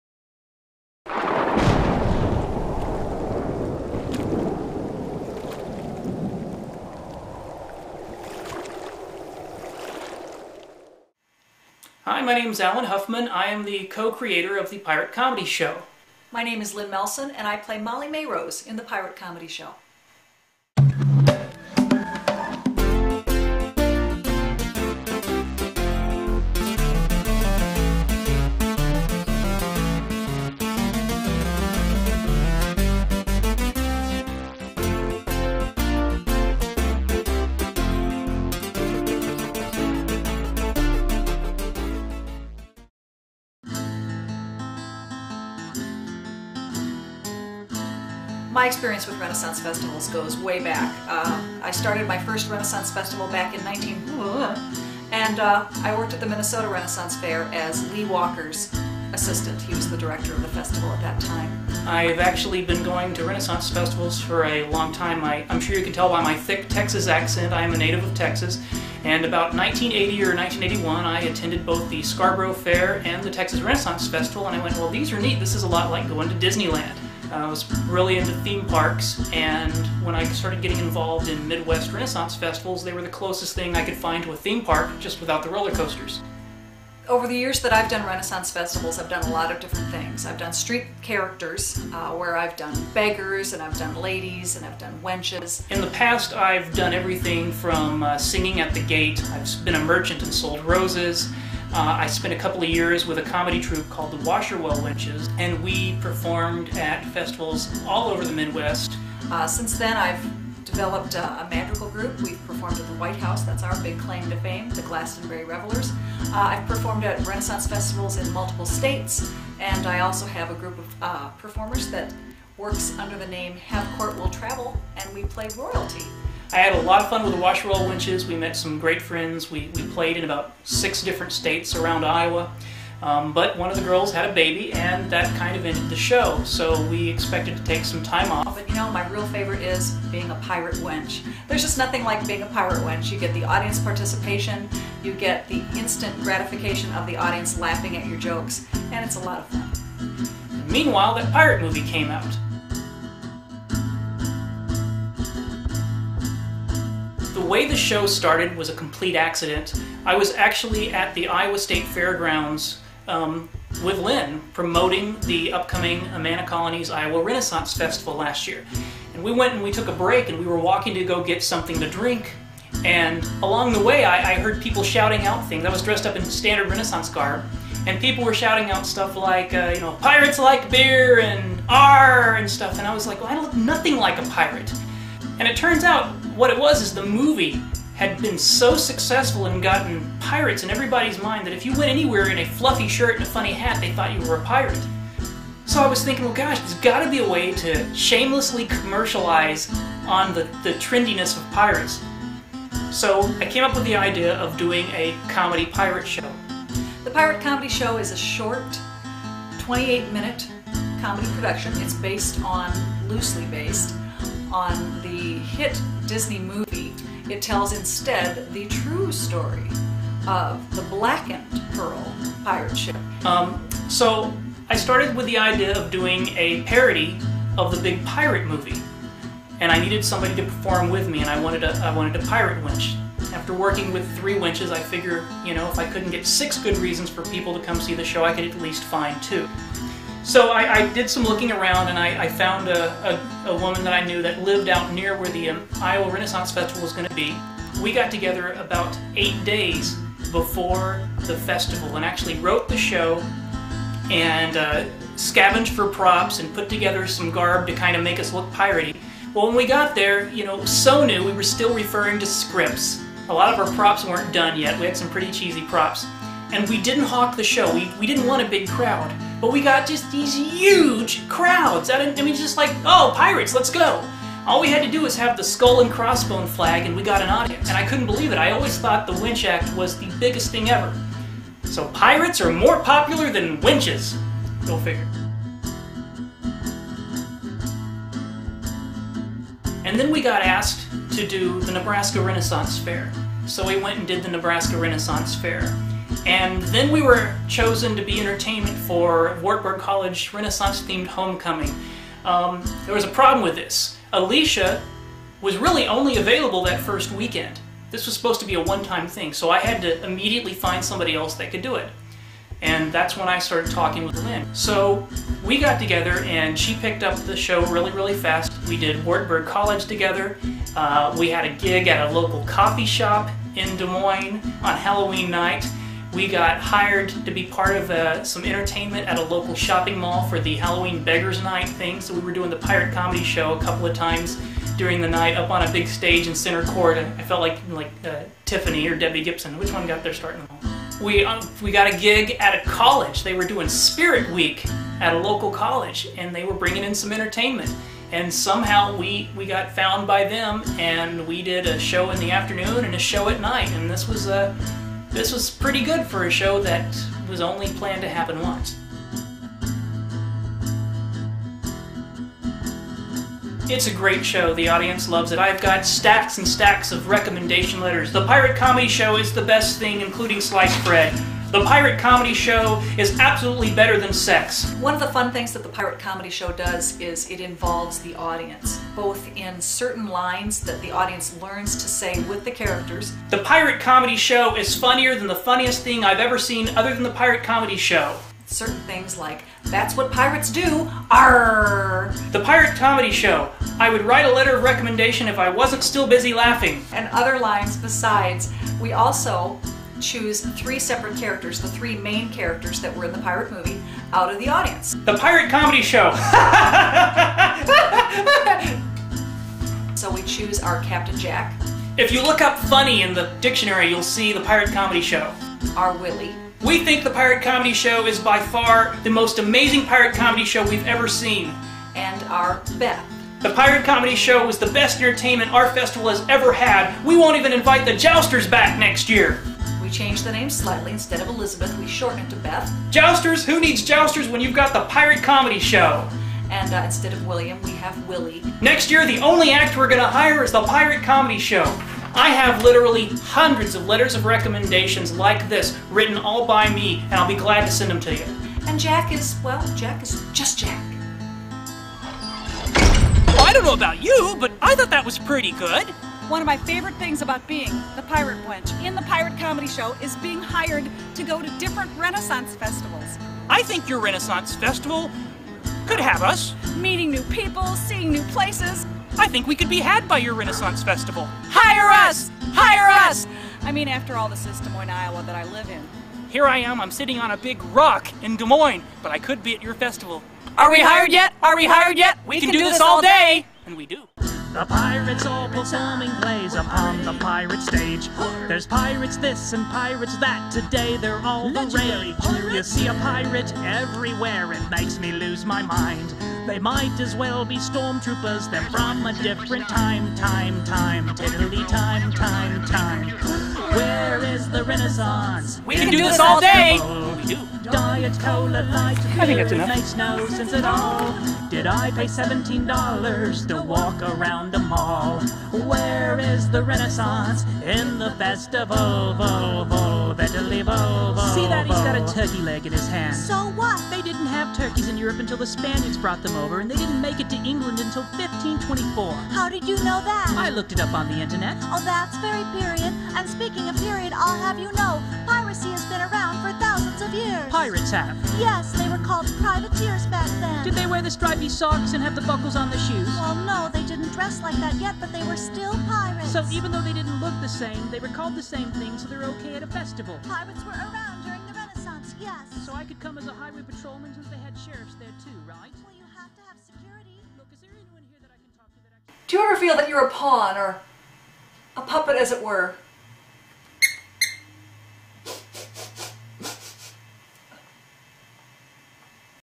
Pirate Comedy Show Promo 2004 (HQ)
This was from a DVD we put together to send to various Renaissance festivals in the region to promote our new comedy show. At this point, the show had only been done twice (eight days after creation at the 2003 Iowa Renaissance Festival, then at the Nebraska Renaissance Faire). Lynne took over for original pirate wench Alicia (who would be unavailable) and we put together new costumes and props.

This video was done to give event producers an idea about "us" as people and to explain the show.

Recorded on the evening of April 29, 2004 in my kitchen in Urbandale, Iowa.